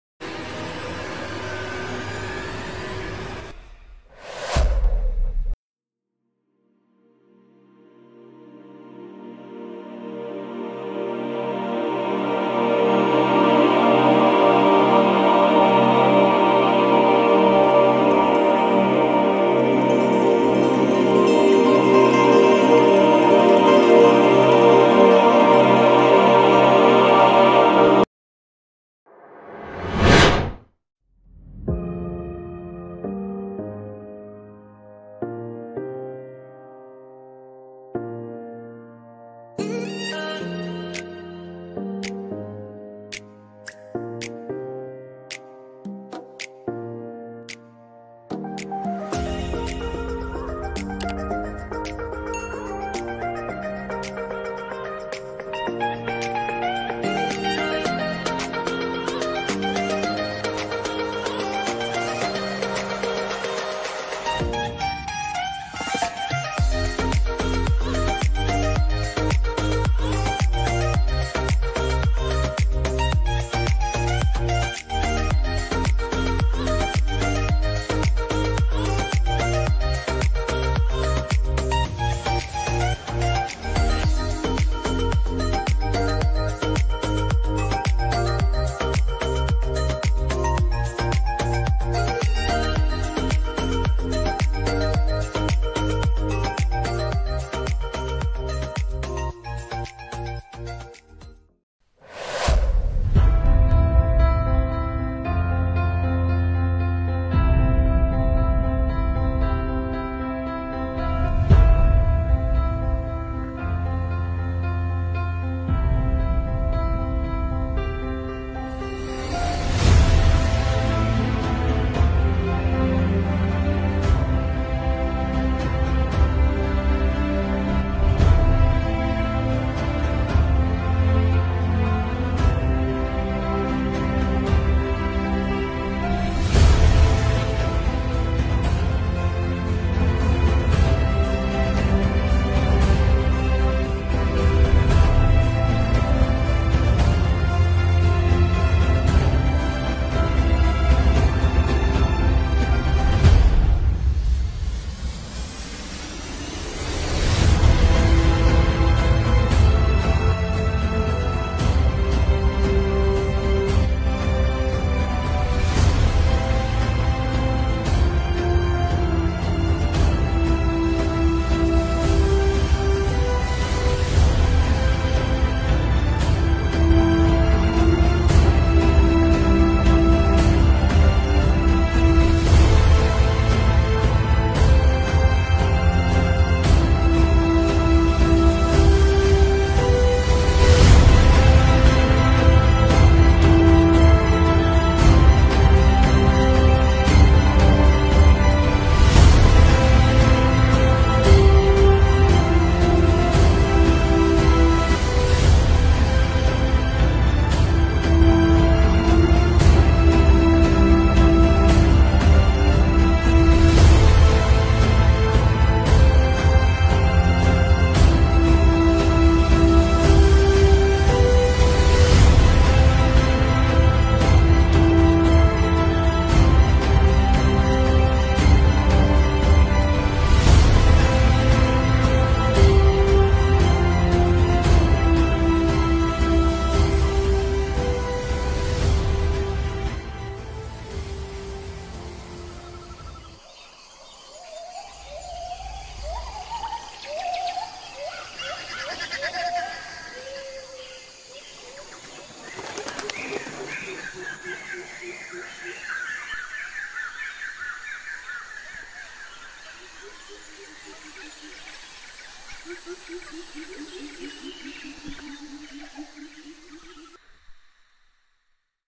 Balong endah ( Taman Nasional Gunung Halimun - Salak )
Balong Endah ini terletak di kawasan Taman Nasional Gunung Halimun Salak - Bogor.
pemandangan di sekitar sini sangatlah bagus,asri dan sejuk loh teman-teman, harga tiketnya pun masih sangat terjangkau serta memiliki parkiran yang sangat luas, di sekitar wisata Balong Endah ini pun banyak penjual makanan jadi jangan takut kelaparan Krn kita tinggal bawa uangnya saja..hehehehe
ayo kita berwisata ke Balong Endah teman-teman.
dan ingat selalu menjaga kebersihan ya terima kasih